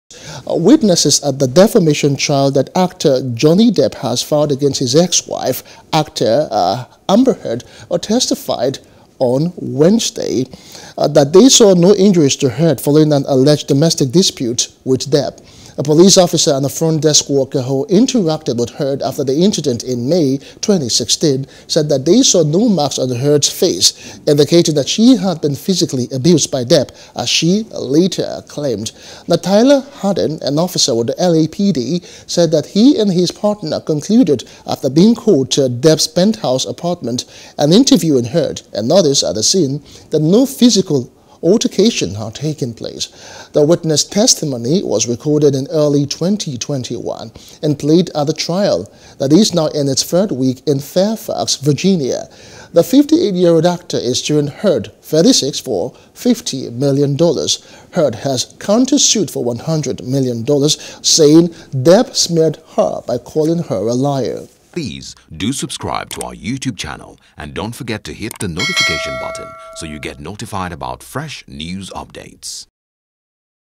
Defamation Trial: Witnesses Say They Saw No Injuries To Amber Heard
Witnesses at the defamation trial that actor Johnny Depp has filed against his ex-wife Actor Amber Heard, testified on Wednesday that they saw no injuries to Heard following an alleged domestic dispute with Depp.